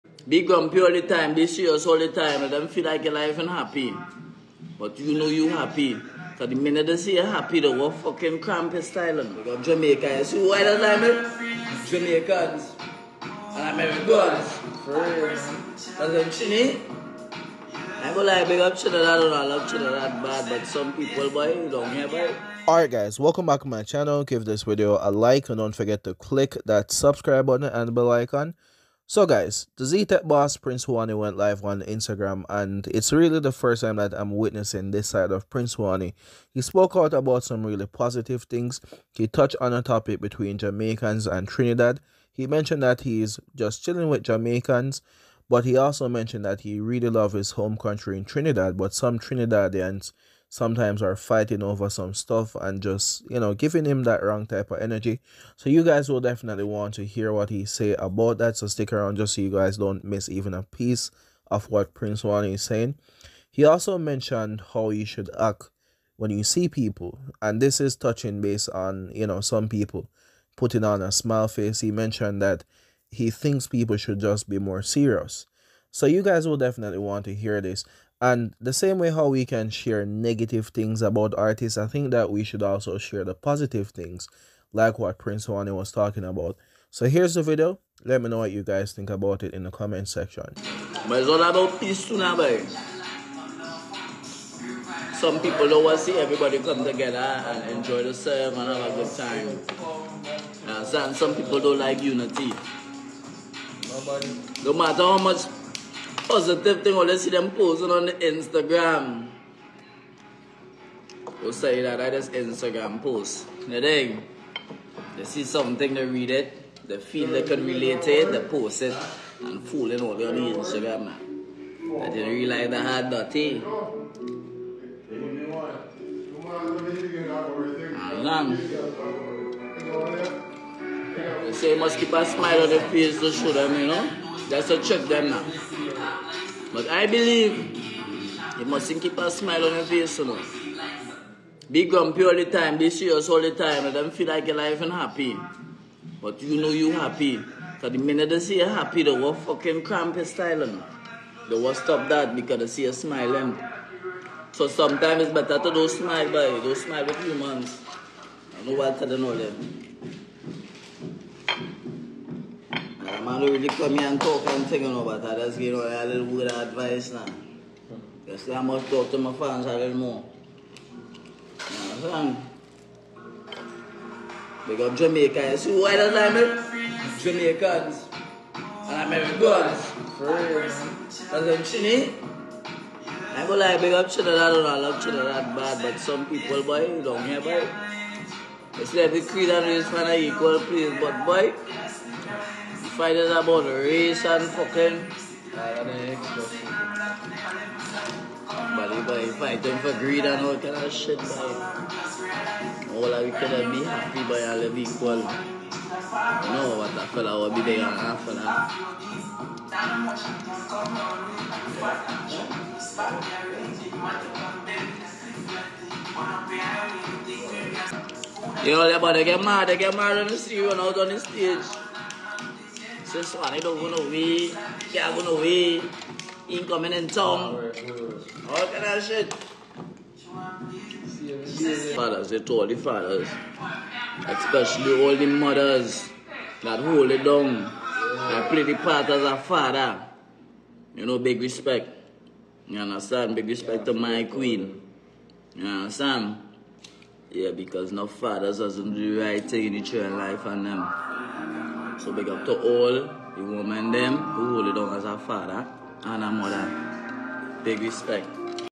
Prince Swanny addresses Linking with Jamaicans vs Trinidadians | Instagram Live | Ztekk
Dancehall Artiste Prince Swanny addresses linking with Jamaicans, Trinidadians, Byron Messia on Instagram Live